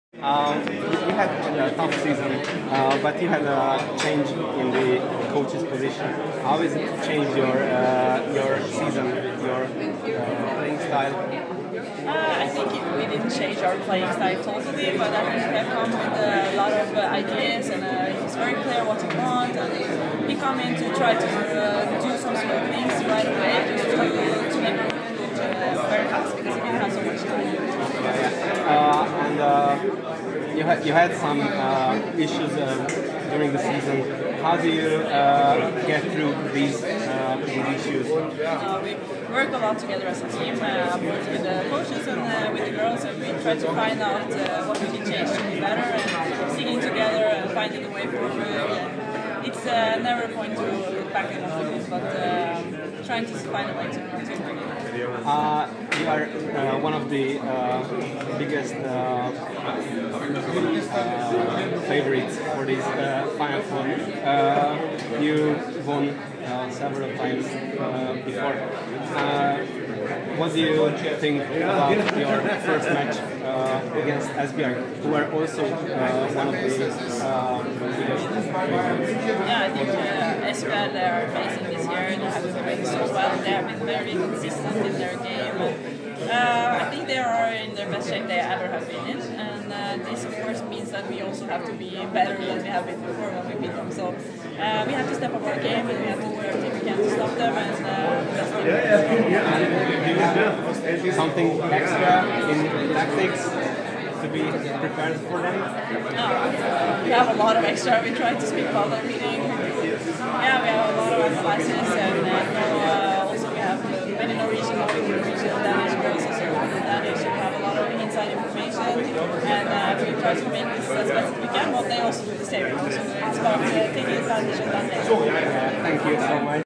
A nulladik napon Kari Brattset, a Győr játékosa válaszolt a Handballexpert kérdéseire
Stábunk is kint volt a budapesti Final Four nulladik napján, ahol játékosokkal, edzőkkel készítettünk rövid interjúkat.